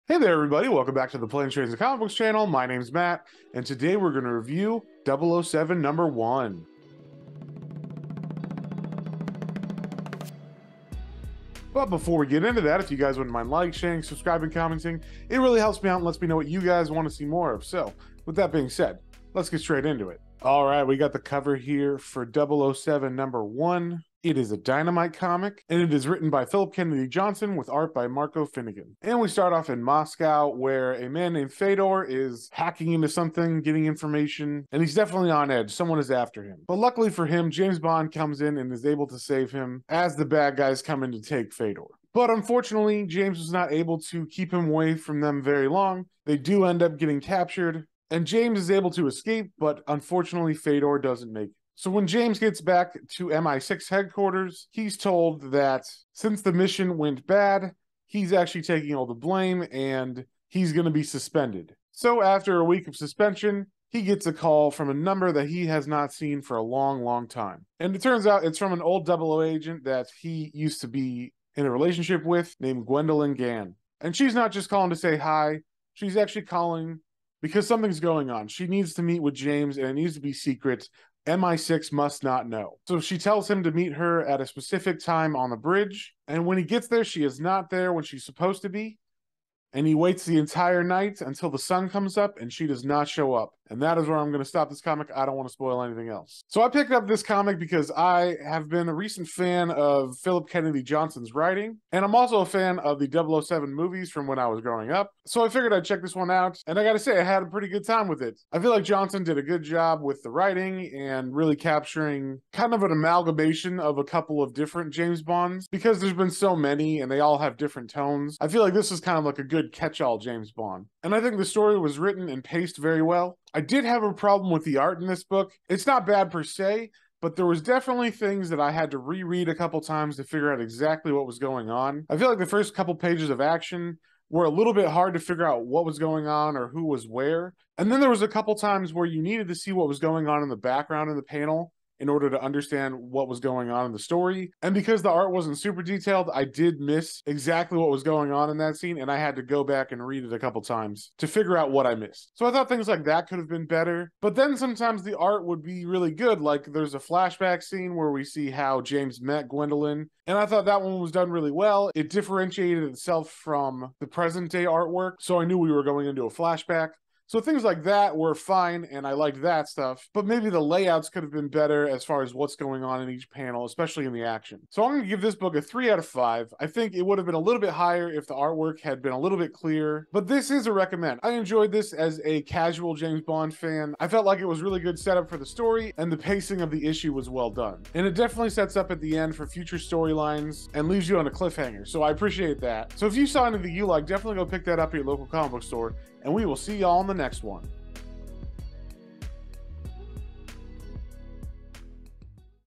COMIC REVIEW || 007 #1
Matt goes through and reviews the new James Bond comic from Dynamite Comics, 007 #1.